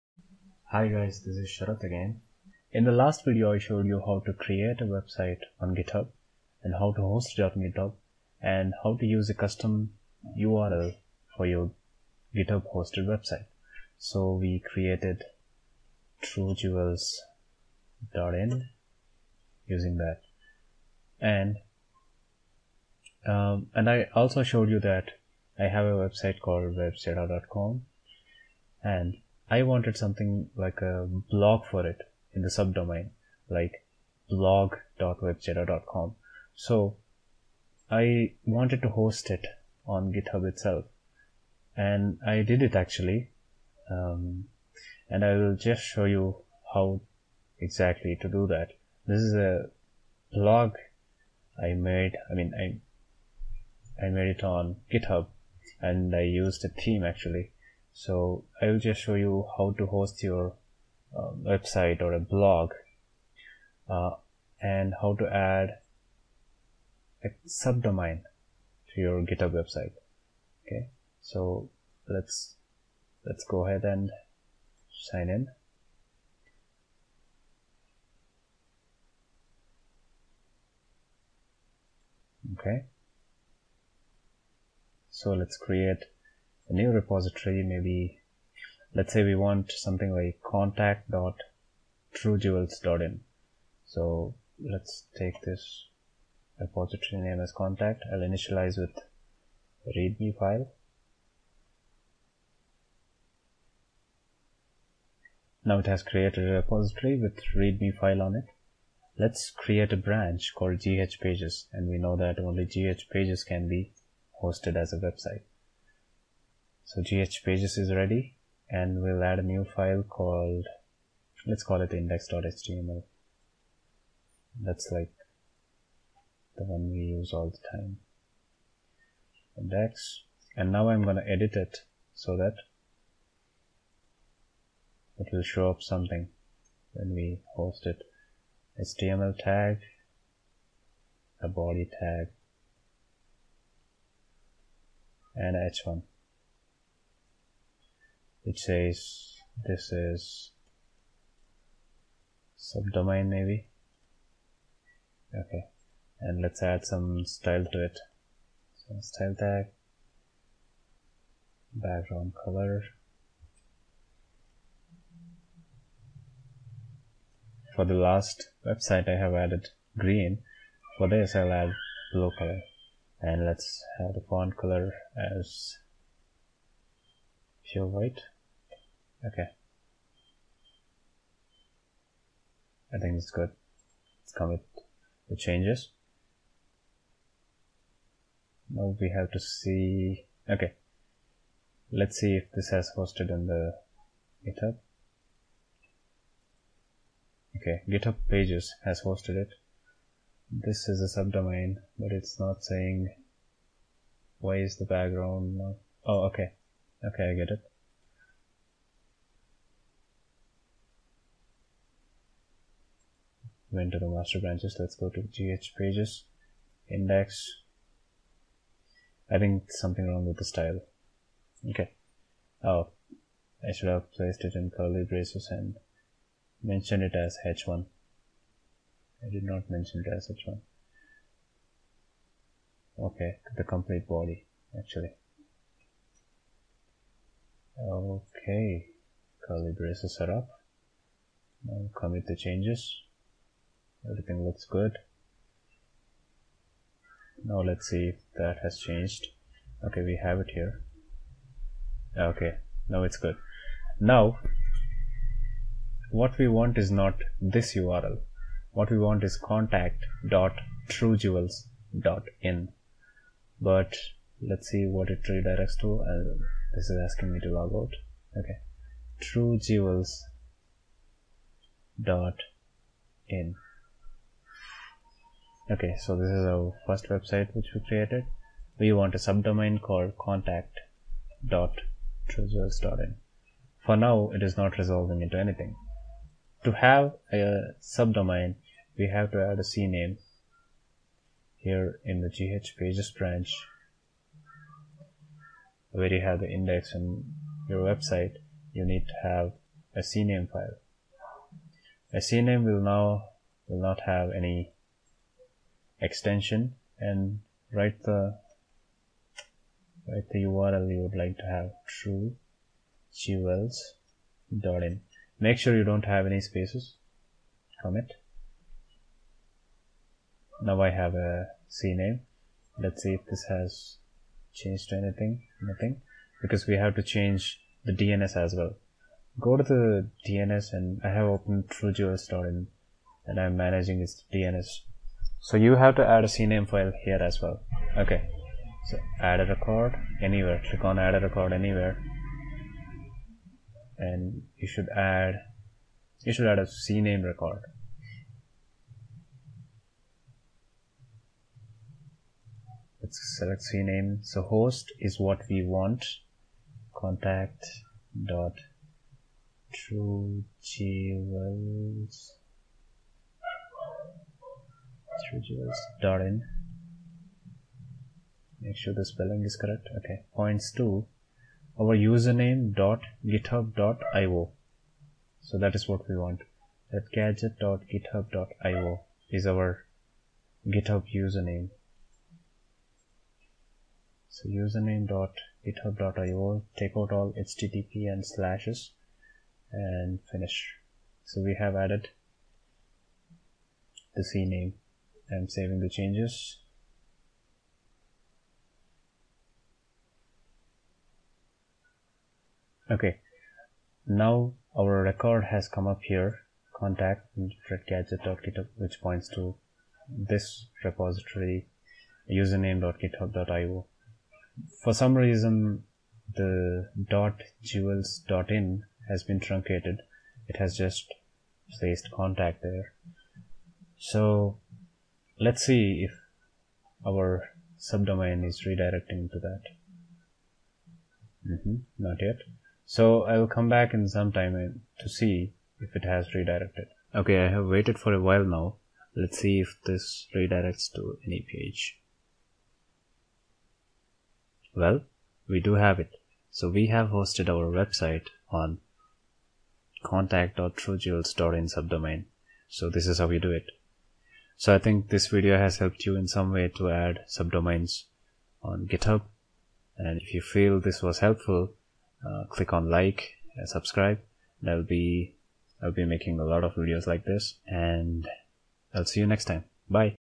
How to add a Subdomain in Github Pages- Tutorial 3
In this video, I'm adding a subdomain to a Github website.

The source code of the website I will be hosting is in the below link

Let's say you have hosted a website called webjeda.com on Github. Now you want a blog for your website. You want to use a subdomain blog.webjeda.com and host it on Github itself. So how to do that?

This video deals with the same problem.

In last two videos, I've shown these steps

And this video deals with adding a subdomain to the website.

I'm using the following

*No CLI or Terminal
*Only html coding

Note: These videos are more focused on web designers and developers.